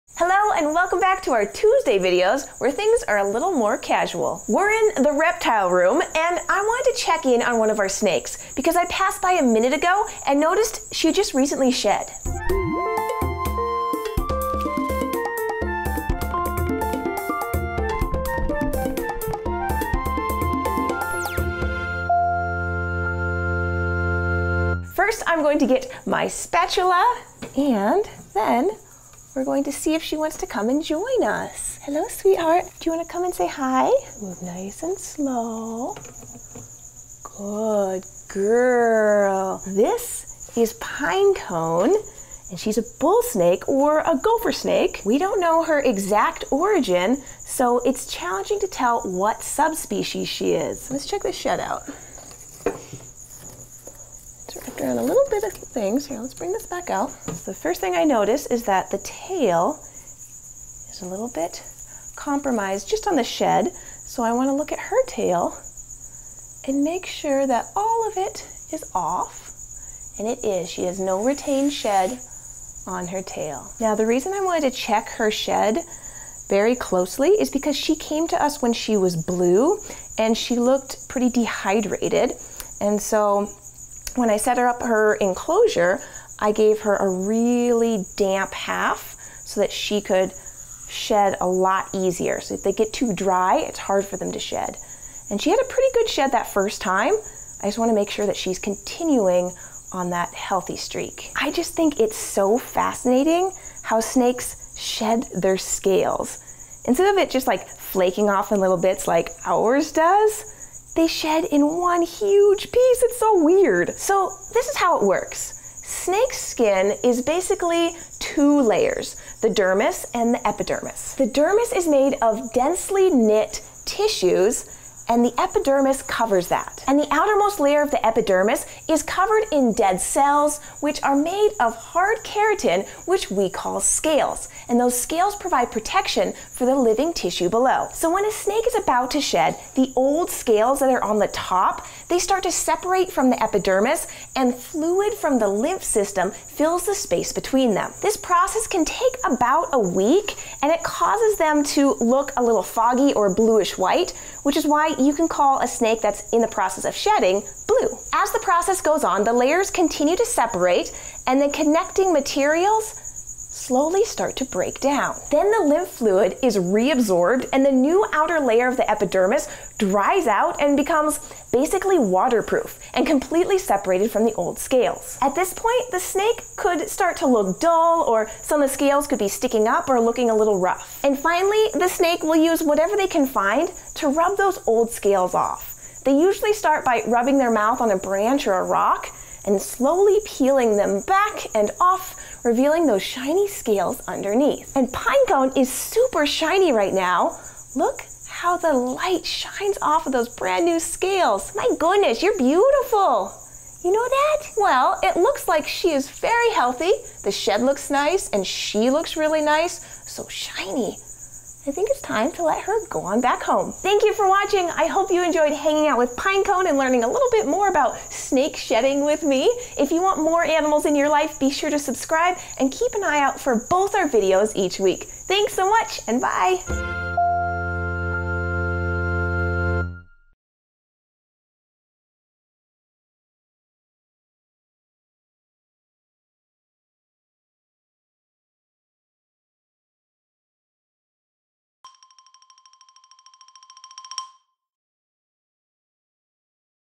How Snakes Shed Their Skin
Jessi checks in on Pinecone the bullsnake to make sure she shed well and is looking healthy now that she's been at Animal Wonders for a few months.

Teresa Whitlock
8bitretomonkey
Xin Ye
Daniel Lumley
Robert Pruitt
david sherman
Mike Pace
Ragnhild
David Polichnia
Corey Dyke
Kevin